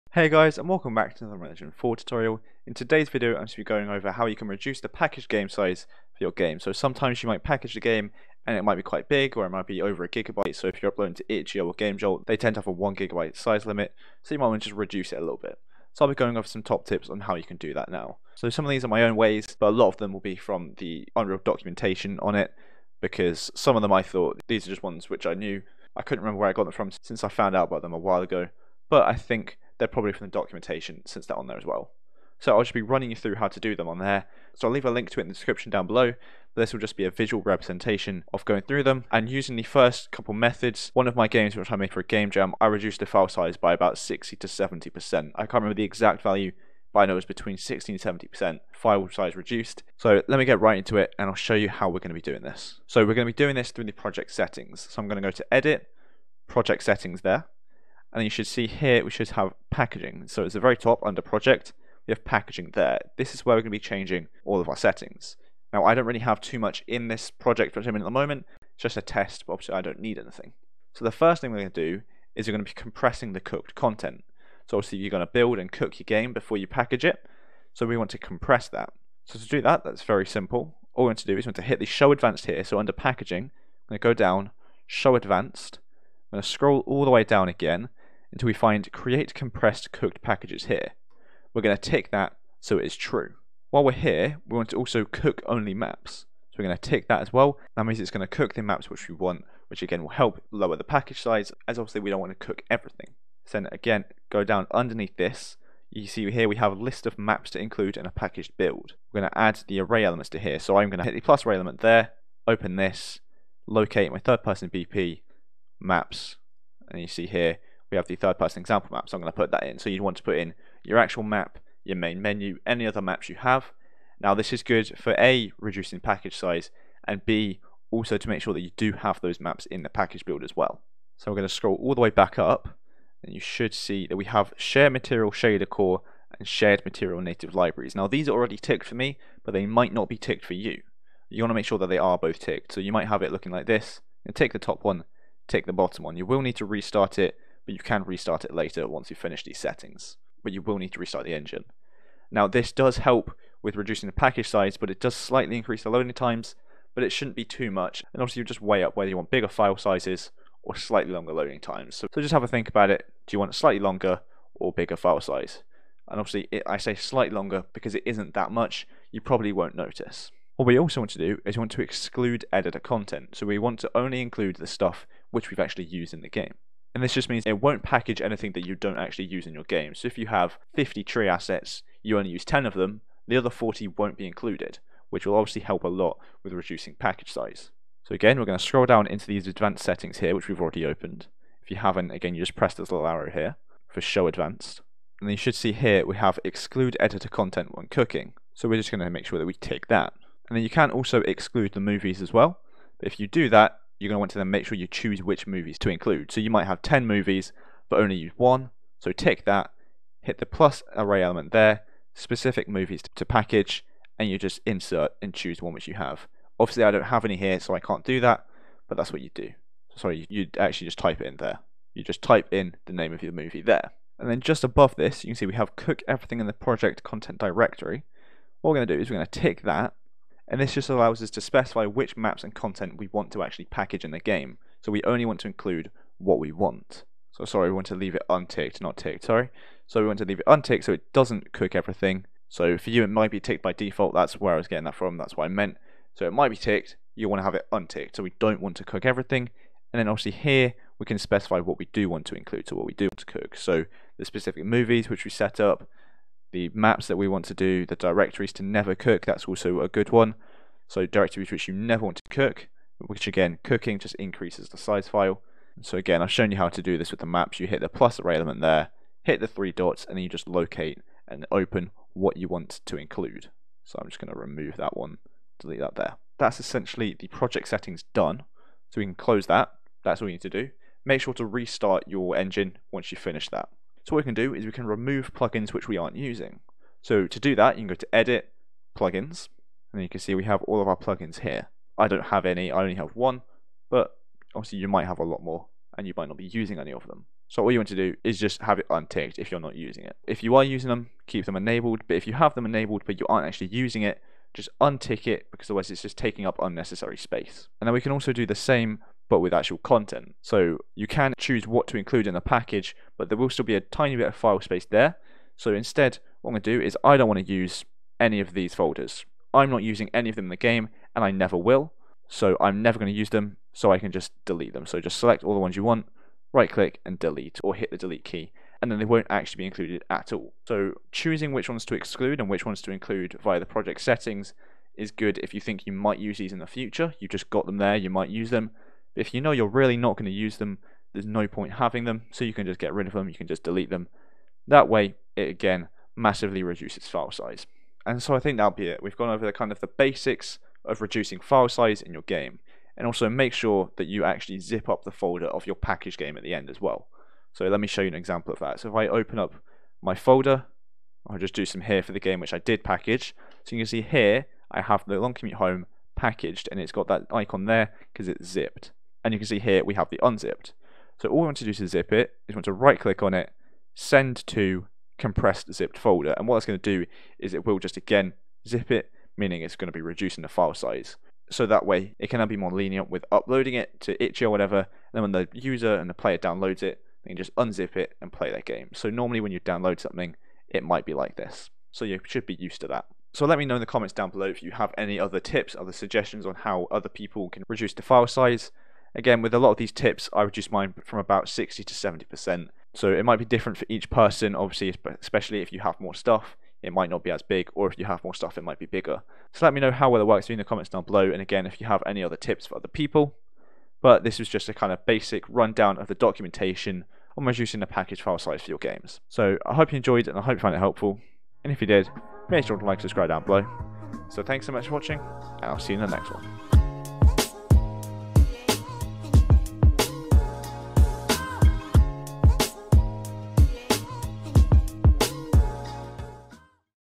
How To Reduce Packaged Game Size | Unreal Documentation - Unreal Engine 4 Tutorial
Hey guys, in today's video, I'm going to be showing you how to reduce the overall file size of your packaged game in Unreal Engine 4. I will be using the unreal docs for most of these methods.

00:00 - Intro/Overview
00:52 - Tutorial
06:33 - Final Overview/Outro